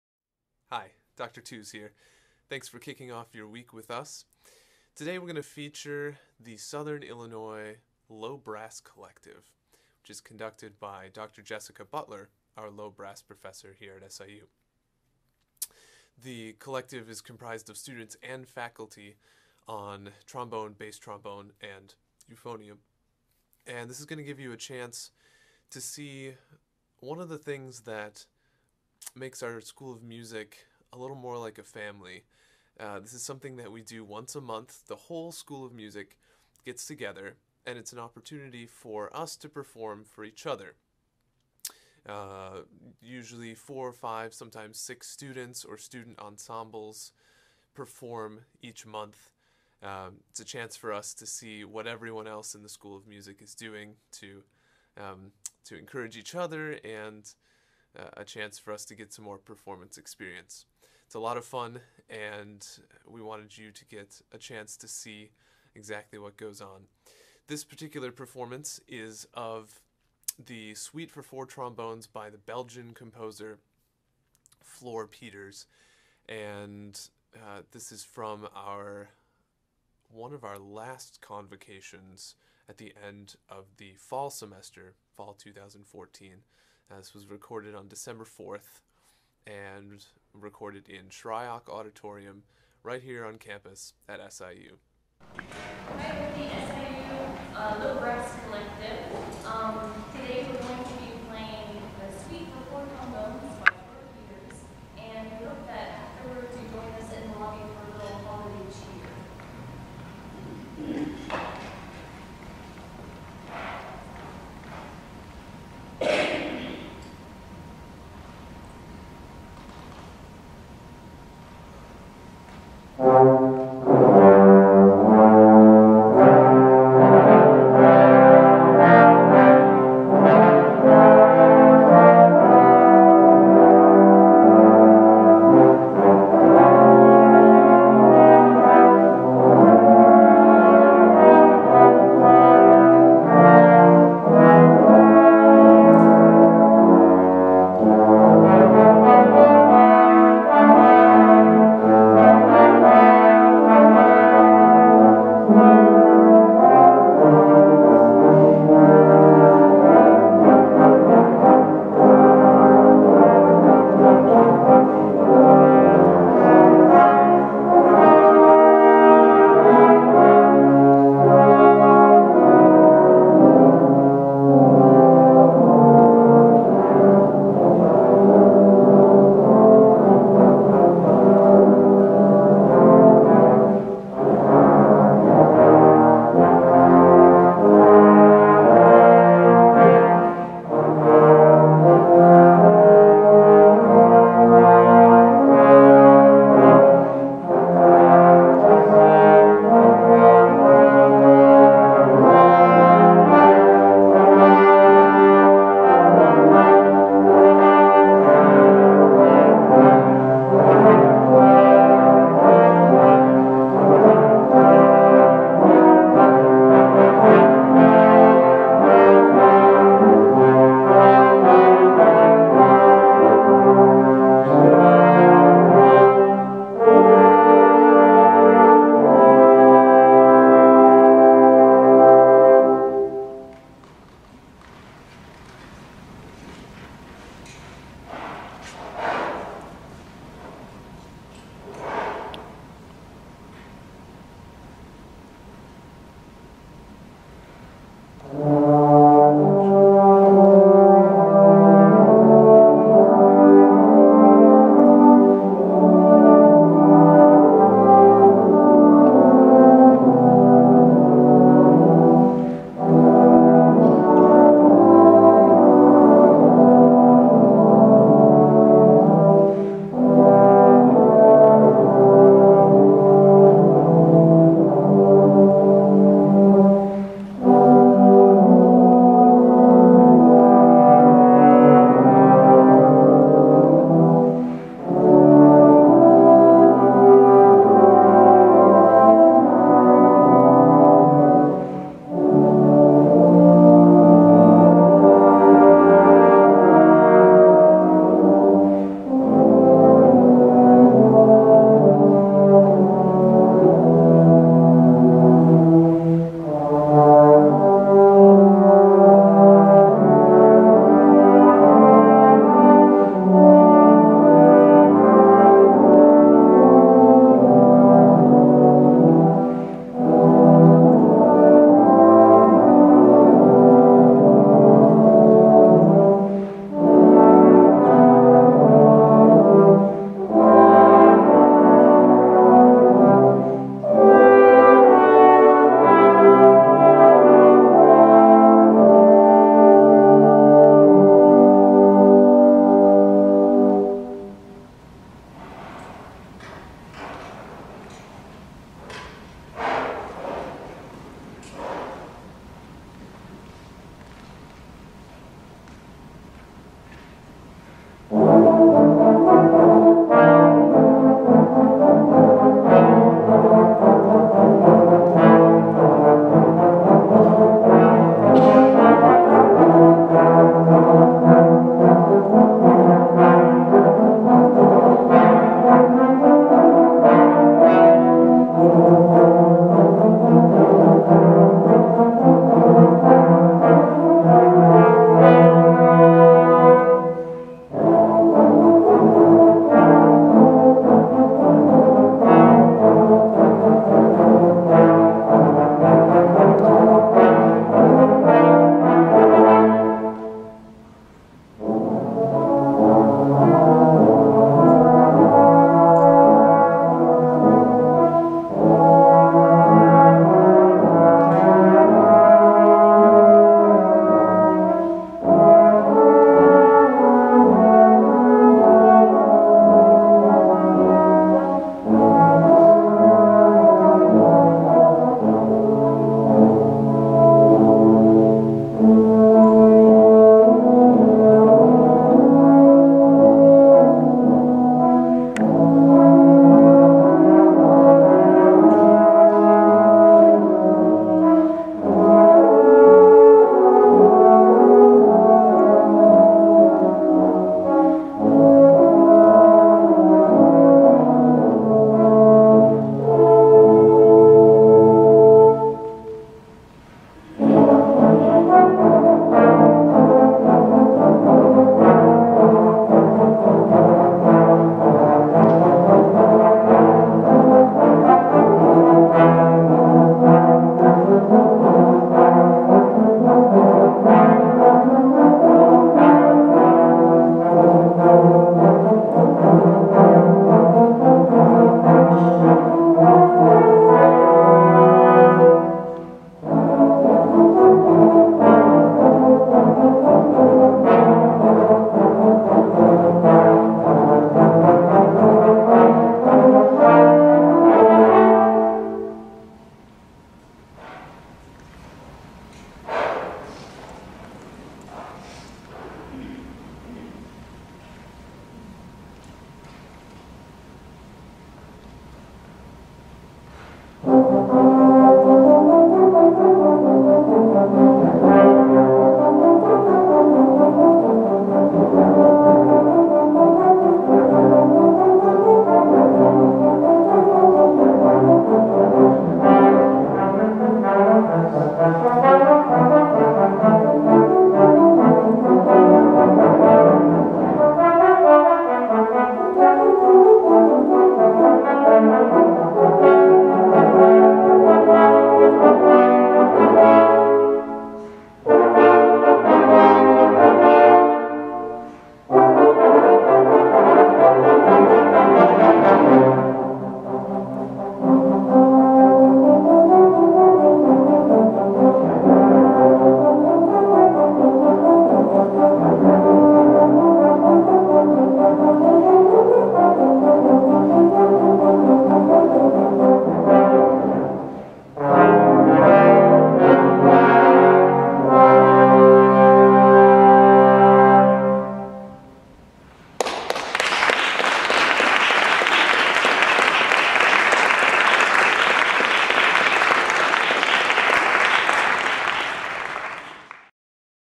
Monday Morning Music #1 (1.26.15): SIU Low Brass Collective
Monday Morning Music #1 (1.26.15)

SIU Low Brass Collective
Dr. Jessica Butler, conductor

Shryock Auditorium
Southern Illinois University Carbondale